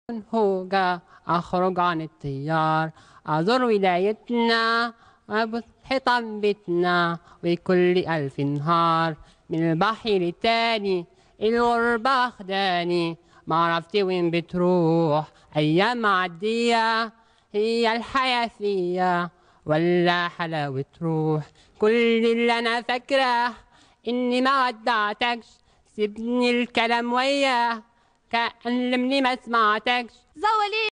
un attardé prend le mic
qnd un attardé essaye de chanté ca donne généralement ca!